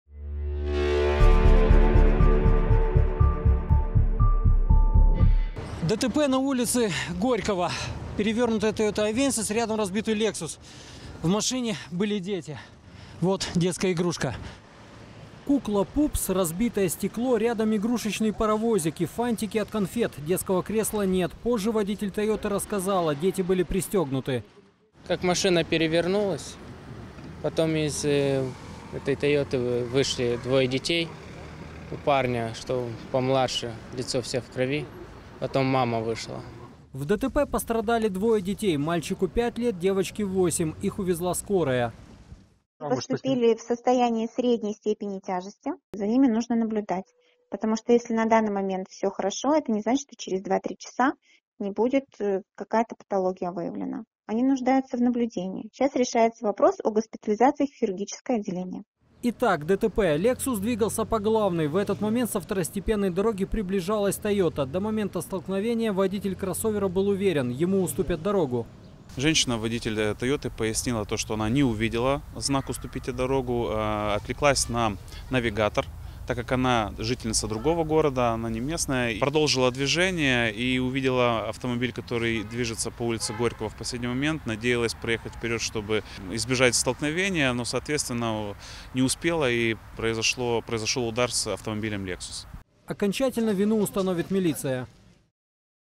ДТП в столице: пострадали дети!
ДТП на улице Горького. Перевернутая «Тойота Авенсис», рядом разбитый «Лексус». В машине были дети. Вот детская игрушка. Кукла - пупс. Разбитое стекло. Рядом игрушечный паровозик. И фантики от конфет. Детского кресла нет. Позже водитель «Тойоты» рассказала: дети были пристегнуты.